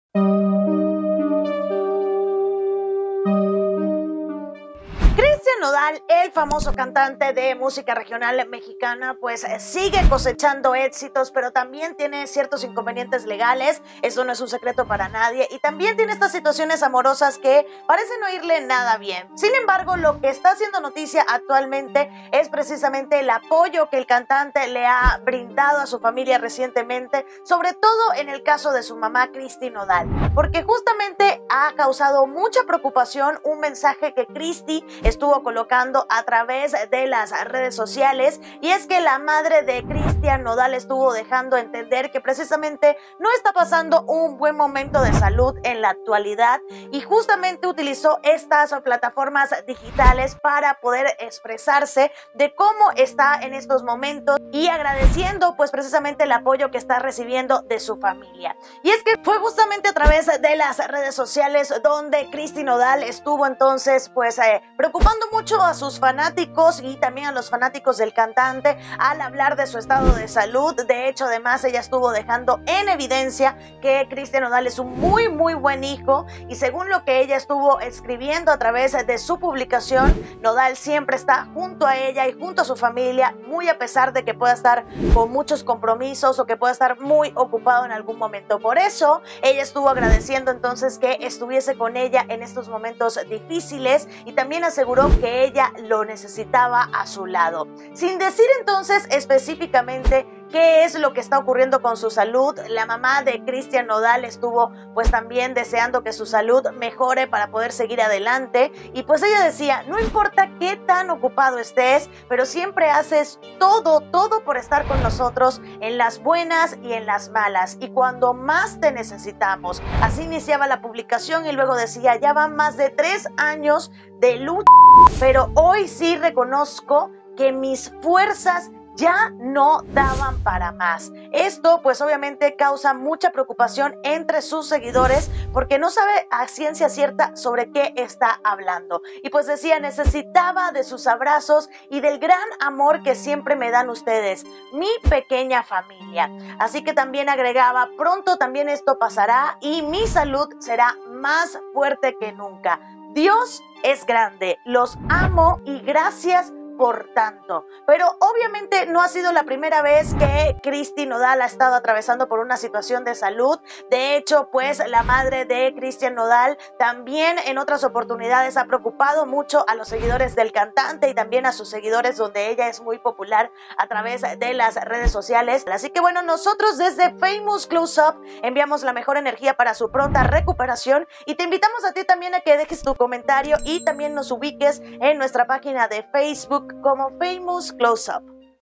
⛔😢 Mamá de Christian Nodal 𝗣𝗥𝗘𝗢𝗖𝗨𝗣𝗔 su estado de salud
Mucho ha sido el misterio que ha rodeado en los últimos tiempos la salud de Silvia Cristina Nodal, la madre de Christian Nodal.